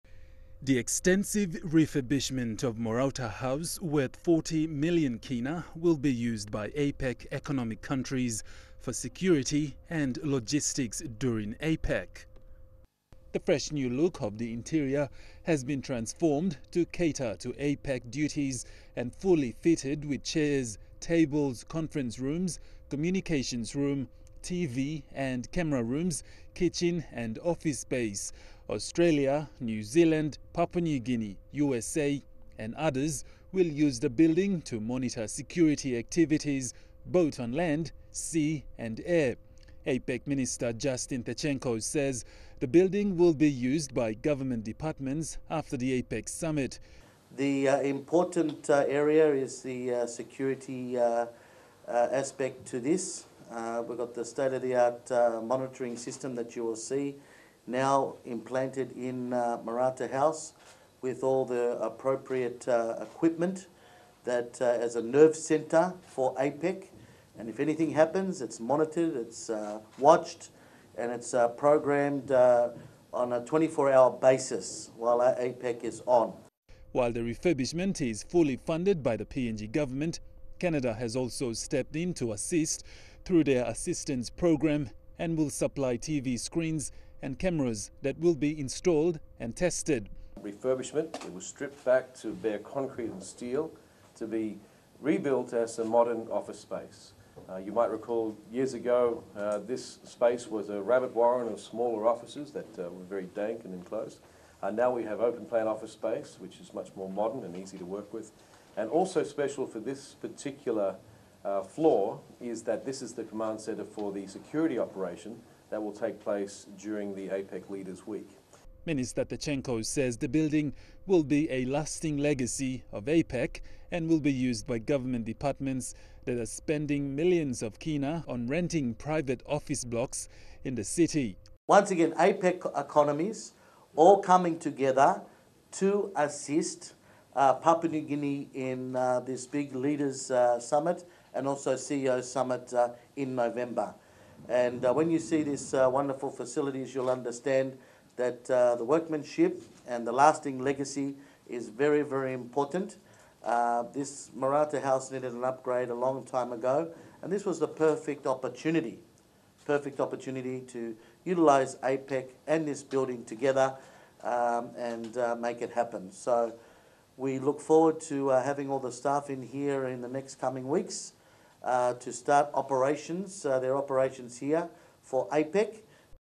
Newly Refurbished Morauta Haus to be used as nerve centre for Security during APEC
APEC Minister, Justin Tkatchenko, and CEO for APEC, Chris Hawkins, gave media a tour of the newly refurbished Morauta Haus in Waigani.

Minister Tkatchenko says the redecorated building will be used by the security departments of APEC member countries as a nerve center for all APEC security and logistical duties.